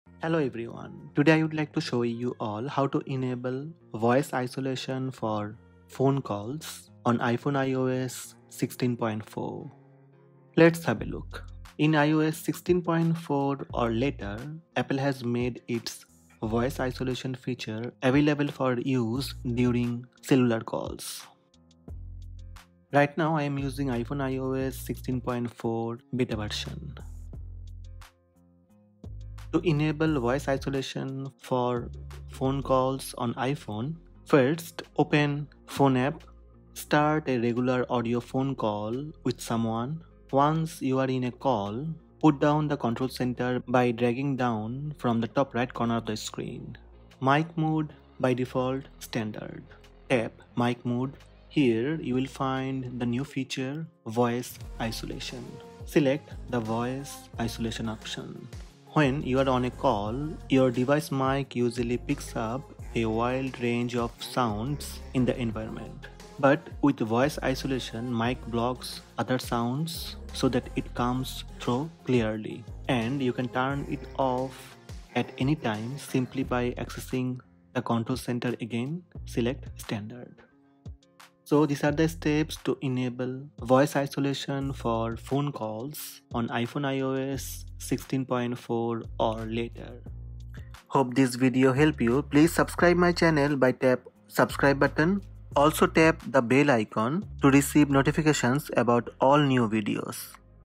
How to Enable Voice Isolation for Phone Calls on iPhone iOS 16.4
How to Enable Voice Isolation for Phone Calls on iPhone iOS 16.4. Apple has made its Voice Isolation feature available for use during cellular calls. When you're on a call, your device's mic usually picks up a wide range of sounds in the environment, but with Voice Isolation, machine learning distinguishes these sounds, blocks out any ambient noise and prioritizes your voice so that it comes through clearly.